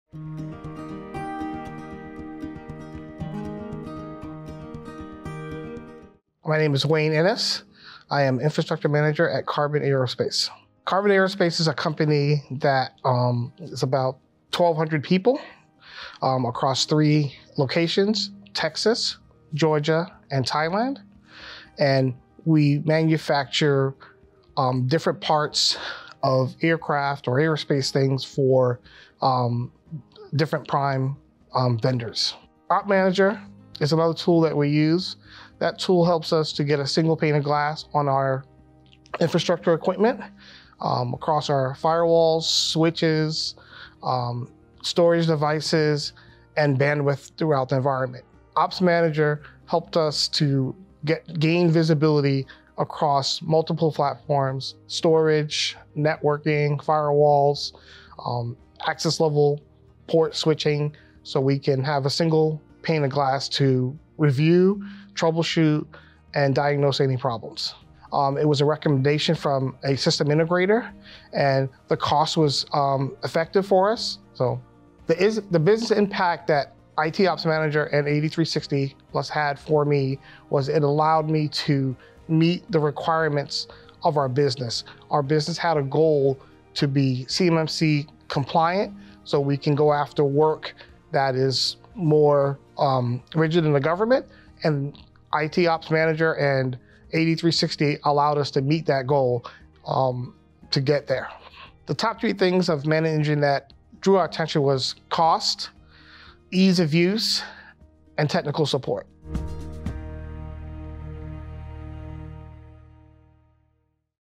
Wayne Inniss Discusses Comprehensive Infrastructure Management with ManageEngine OpManager
In this video, Wayne Inniss, Infrastructure Manager at Qarbon Aerospace, shares how ManageEngine OpManager has revolutionized their infrastructure management. Discover how OpManager provides a single pane of glass for monitoring their infrastructure equipment, including firewalls, switches, storage devices, and bandwidth across their environment. Learn how OpManager offers comprehensive visibility across multiple platforms, enhancing their ability to review, troubleshoot, and diagnose issues efficiently. Don't miss this insightful discussion on optimizing infrastructure management with ManageEngine OpManager!